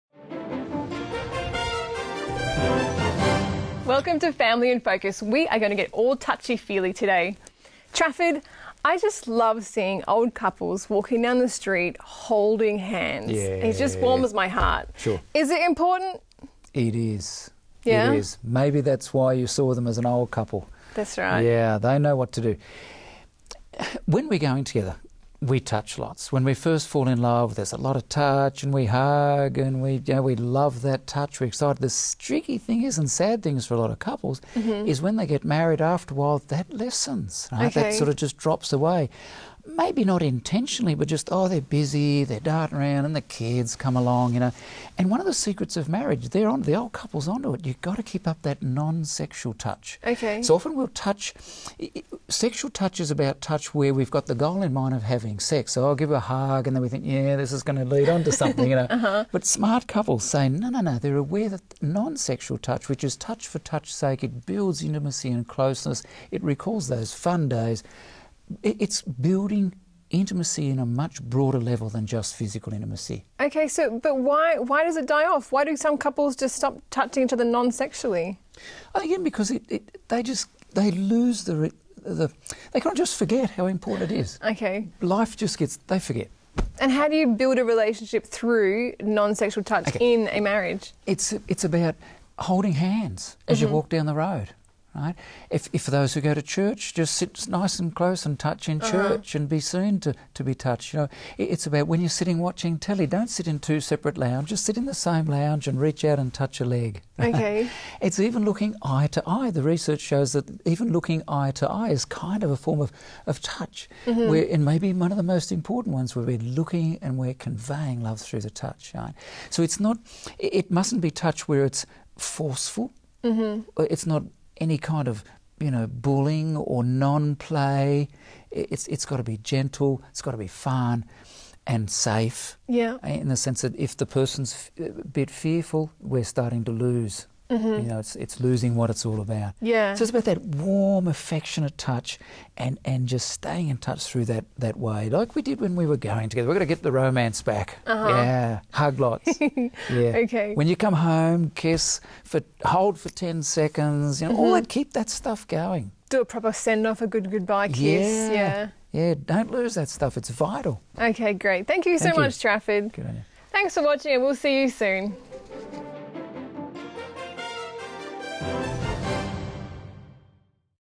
The importance of touch
In a healthy and lasting marriage it's important that physical touch is more than a prelude to sex.